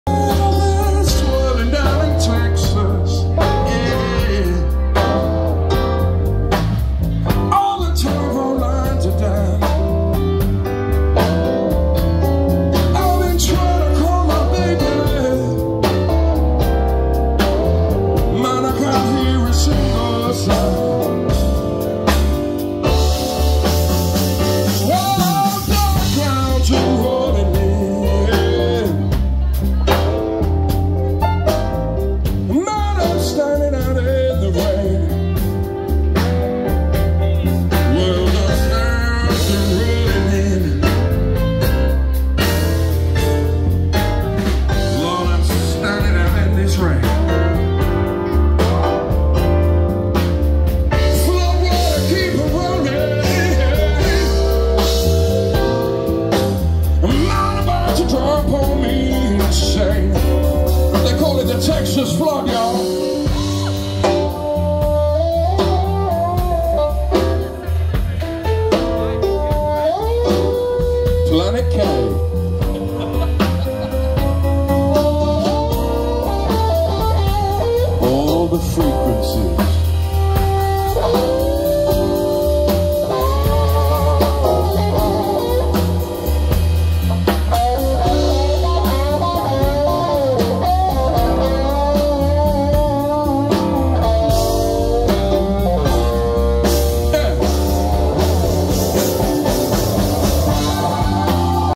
Pat Fulgoni Blues Experience at The Owl Inn Bank Holiday Festival (3)
The Owl Inn Rodley, Leeds
25 August 2024